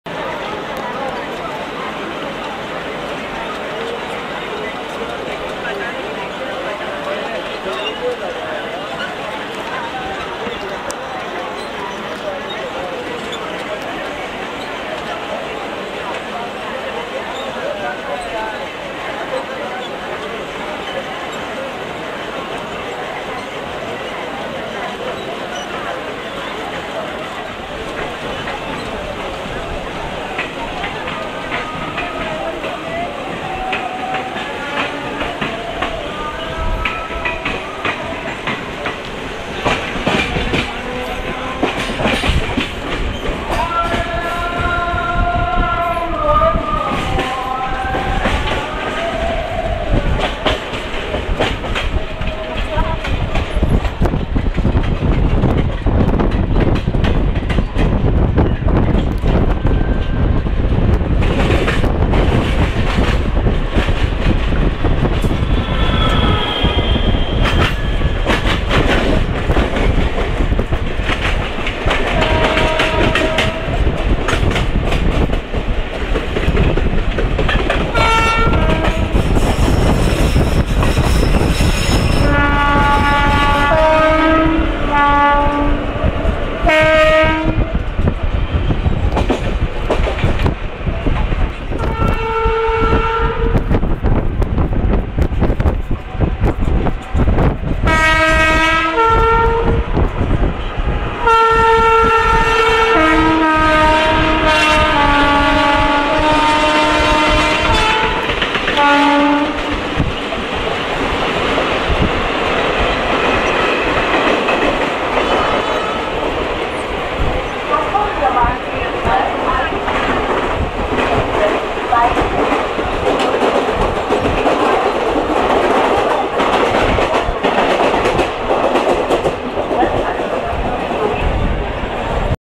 Parallel racing between WAP5 hauled Flying Ranee and MRVC EMU
12921 BCT-ST Flying Ranee SF Exp departs from ADH hauled by BRC WAP5 30062 and within few seconds my MRVC EMU also departs and does a superb parallel racing.

Enjoy the WAP5 and Siemens MRVC EMU acceleration.